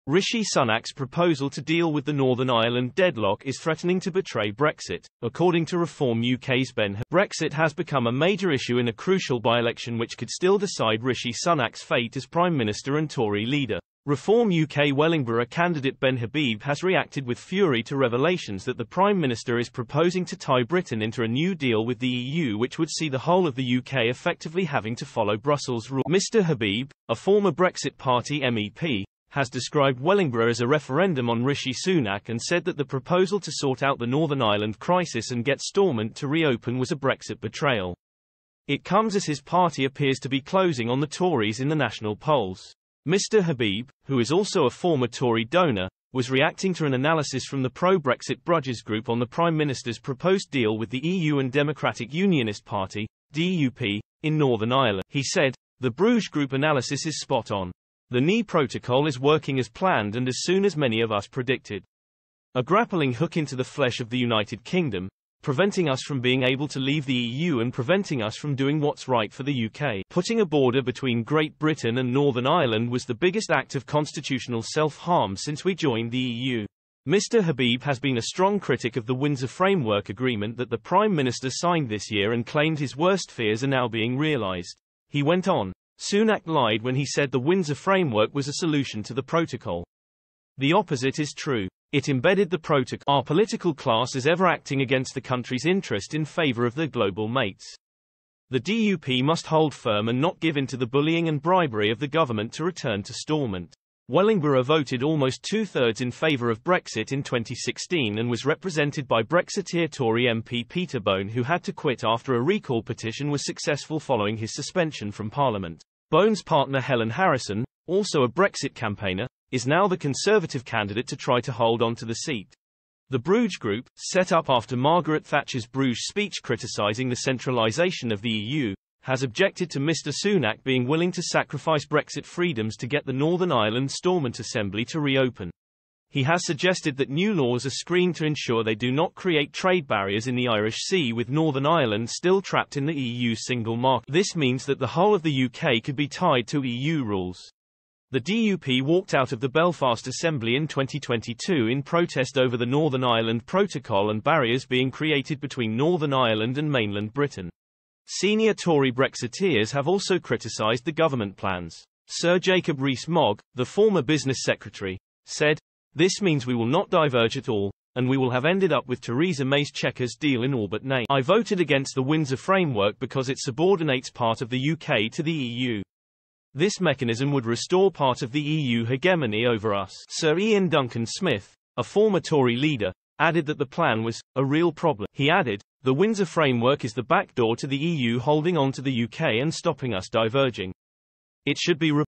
'Brexit betrayaI' warning with Rishi Sunak's new deaI THREATENlNG to tie UK to EU forever
Brexit has become a major issue in a crucial by-election which could still decide Rishi Sunak's fate as Prime Minister and Tory leader.